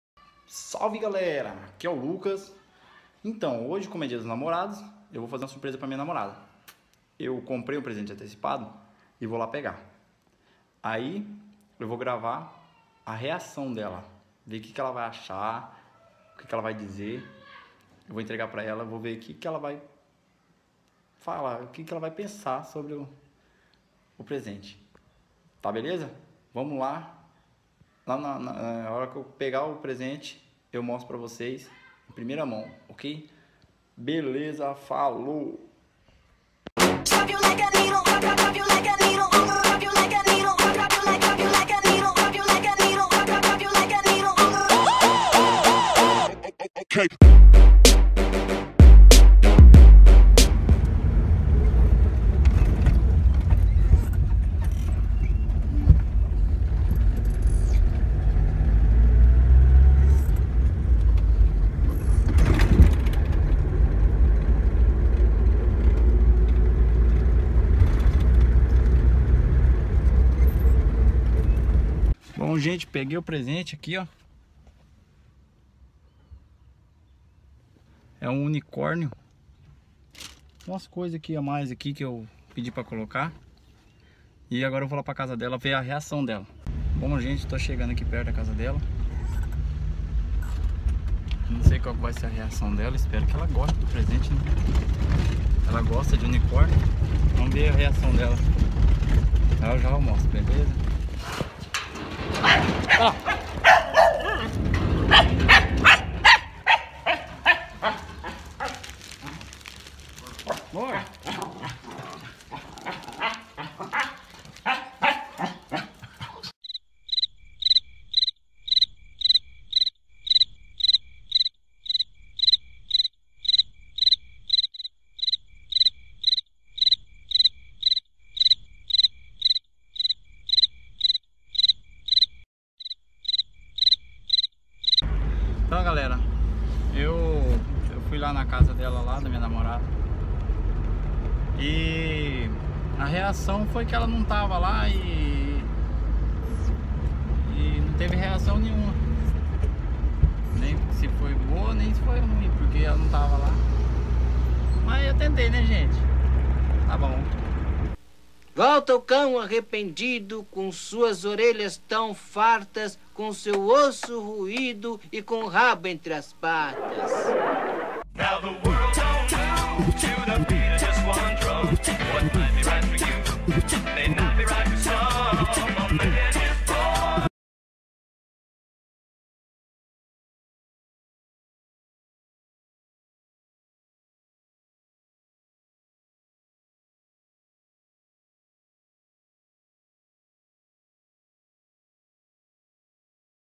leia a discrição ! fui fazer uma surpresa deu ruim !!
acabou que por um imprevisto não saiu da forma esperada ...
mais o importante é oque sentimos e oque eu sinto por você único te amo meu amor ! (( se esse video chegar a 50 like eu posto a surpresa que ela fez pra mim))